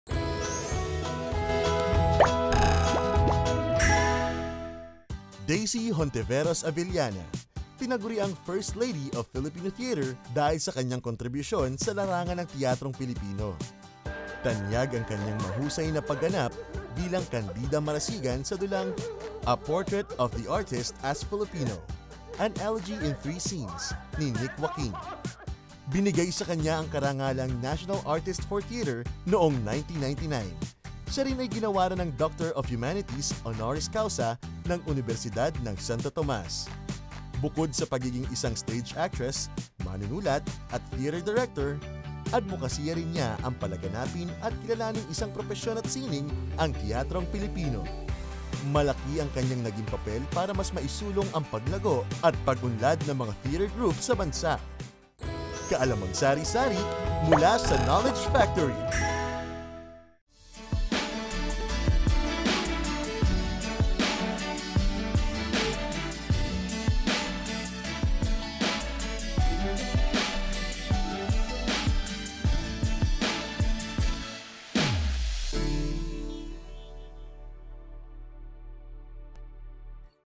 Daisy Hontiveros Avellana | National Artist of the Philippines | Knowledge Factory
Dahil ang February ay ArtsMonth, let's give the spotlight sa ilan sa ating National Artists! Mahilig ba kayong manood ng mga dula? Kilalanin si Daisy Hontiveros Avellana, ang "First Lady of Philippine Theater".

3rd Floor Main Building, ABS-CBN Compound, Sgt. Esguerra Ave., South Triangle, Diliman, Quezon City